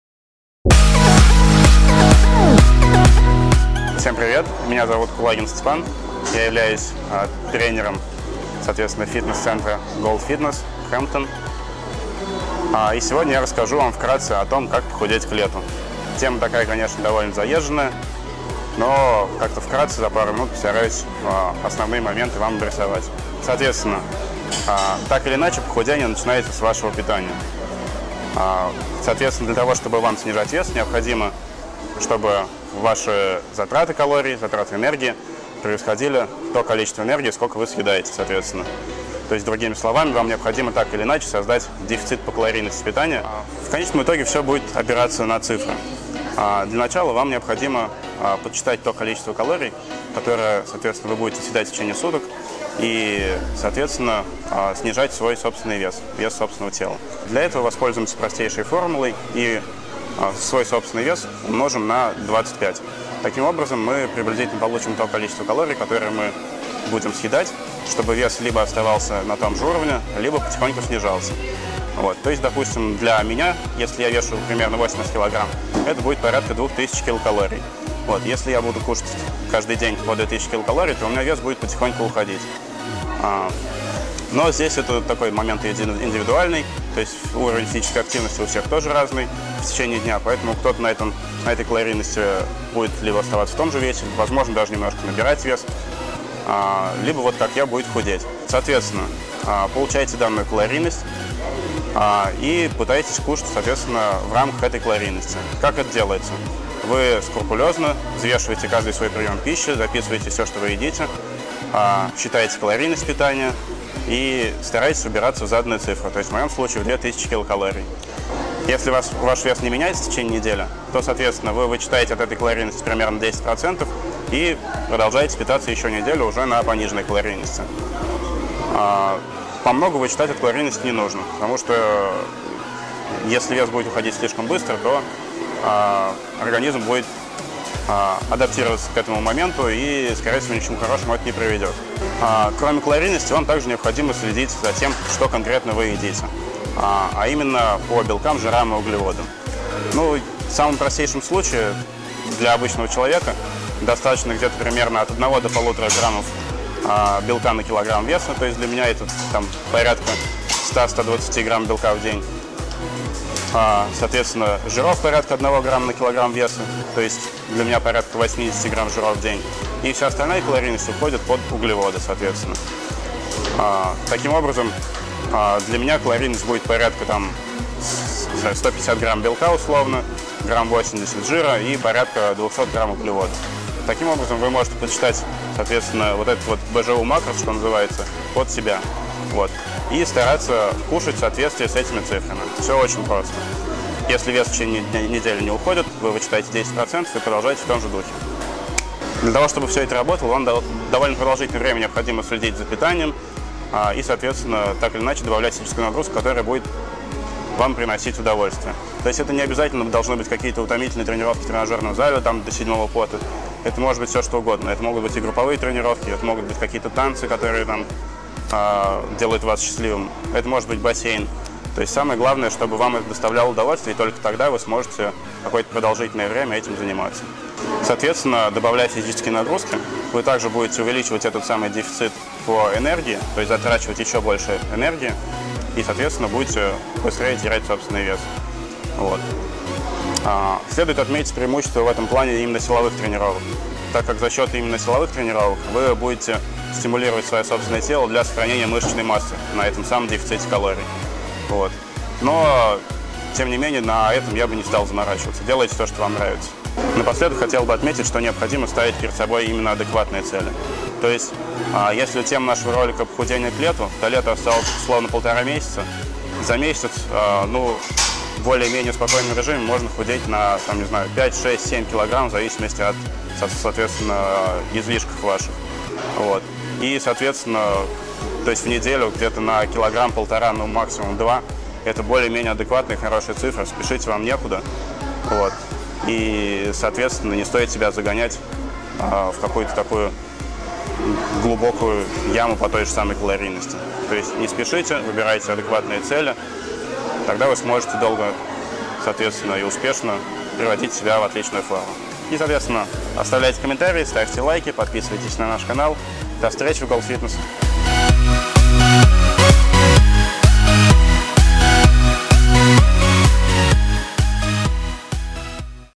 Как похудеть к лету? Советы от Степана Кулагина
Сегодня вы узнаете, как похудеть к лету. А расскажет вам об этом Степан Кулагин.